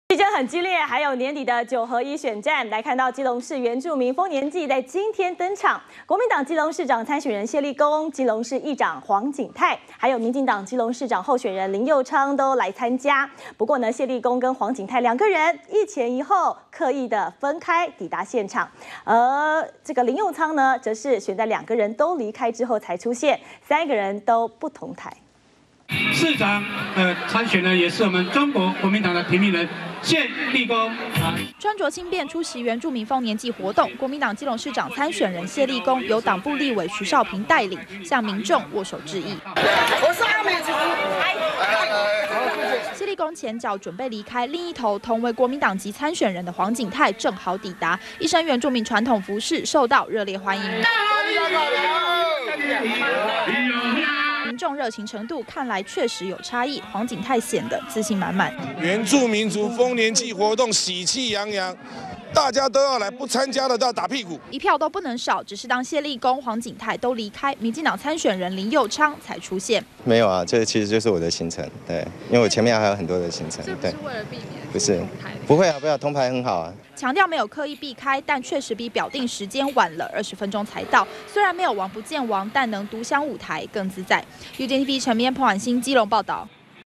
【2014.08.23】基市長三參選人 同場拜票不同台 -udn tv
基隆市原住民豐年季今天熱鬧登場，國民黨基隆市長參選人謝立功、基隆市議長黃景泰與民進黨基隆市長候選人林右昌，都特別參與，只是謝立功與黃景泰兩人，一前一後抵達現場，擦身而過沒有互動林右昌則是，選在兩人都離開了才出現，三人都不同台。完整內容 , FB , Twitter , G+